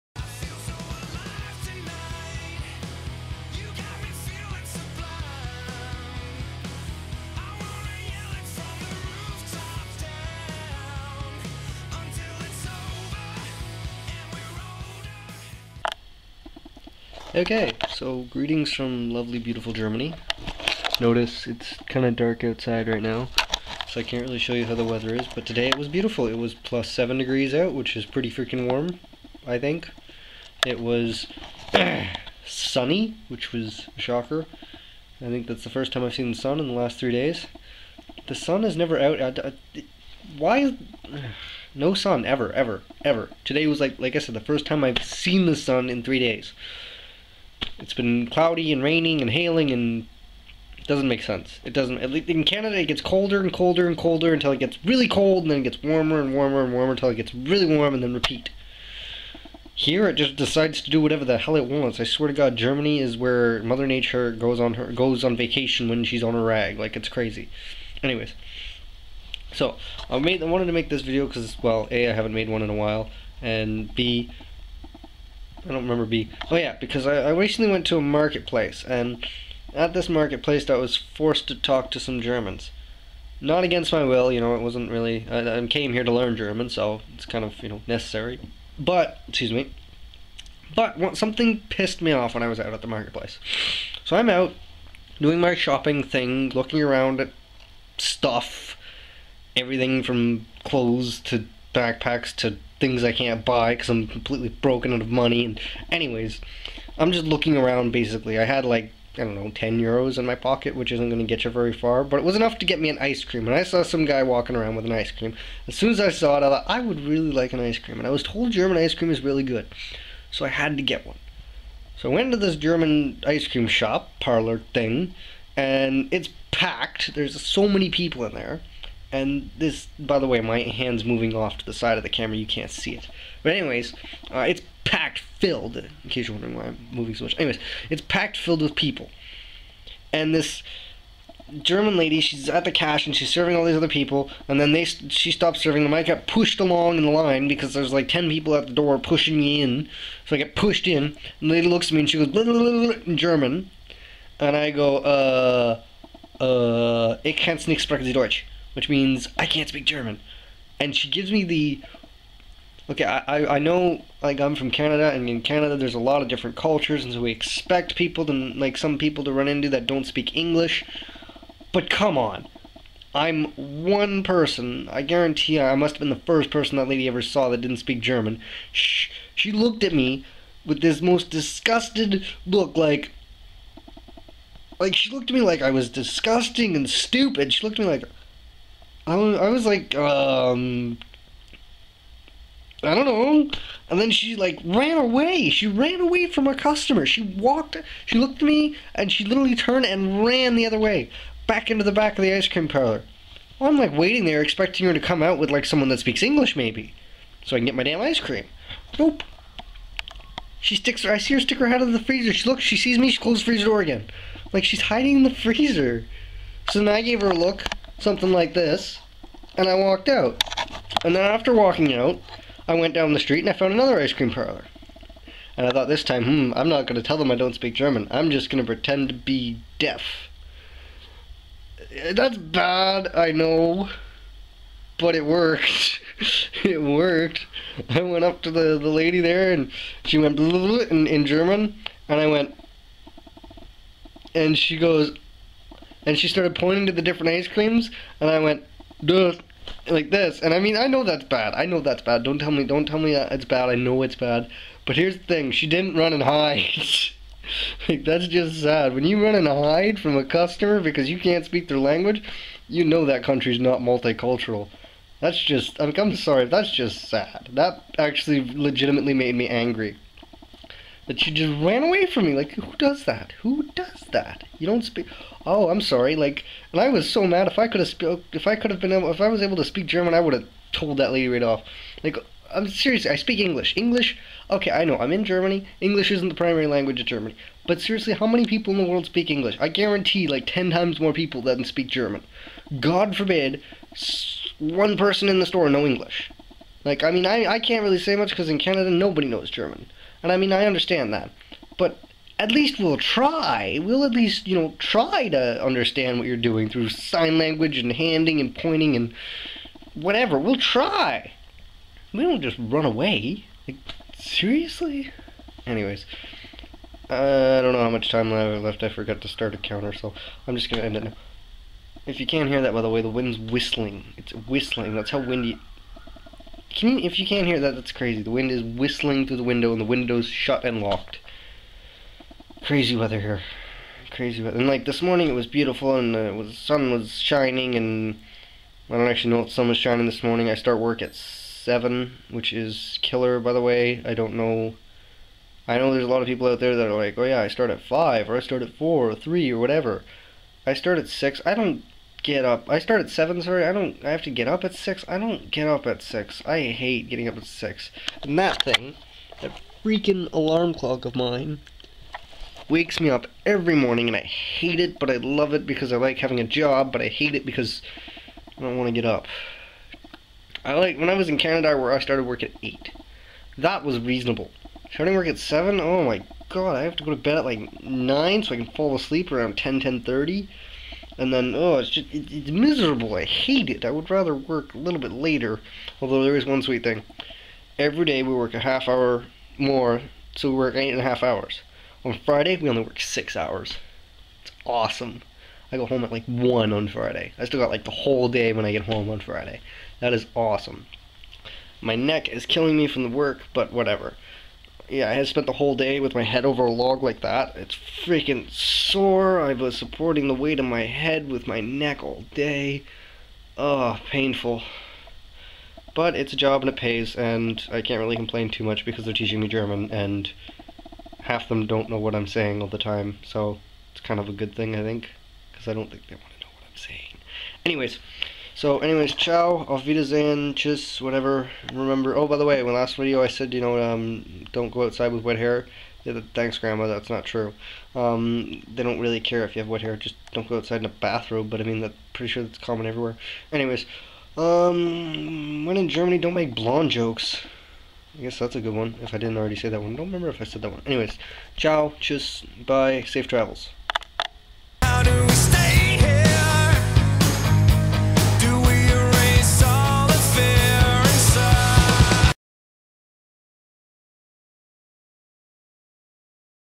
Entry 5 Talking to Germans
what happened when i tried to buy icecream in germany